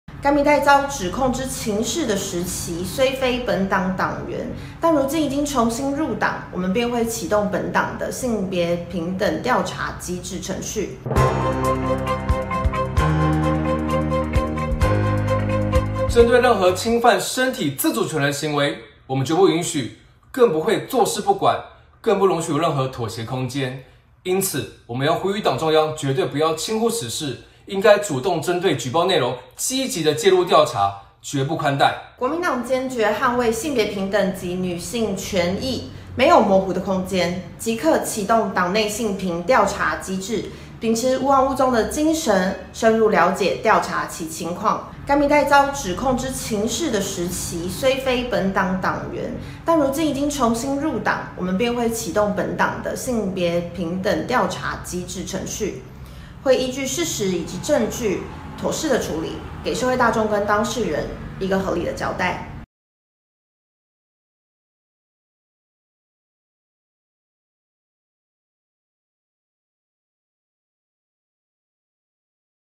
傅崐萁遭控性騷時非黨員 國民黨:重新入黨就會調查
媒體人董成瑜指控國民黨立委、時任花蓮縣長傅崐萁9年前曾對她性騷擾，國民黨昨天表示，將深入調查瞭解相關情況，會依照事實及證據妥適處理。不過，當時傅崐萁是遭國民黨開除黨籍的狀態，但國民黨表示，傅已重新入黨，便會啟動性平調查機制。